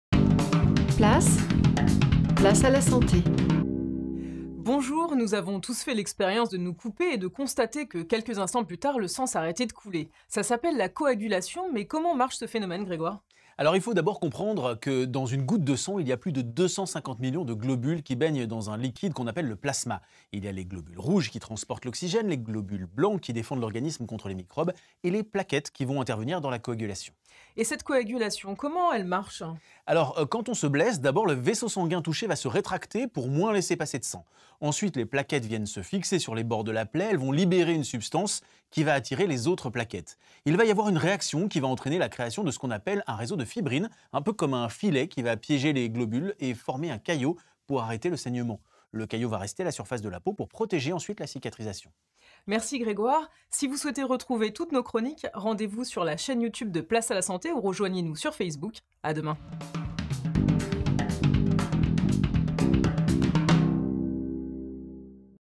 Pourquoi le sang s'arrête de couler après quelques instants lorsque l'on se coupe ?
Place à la santé : Les Bobos N°3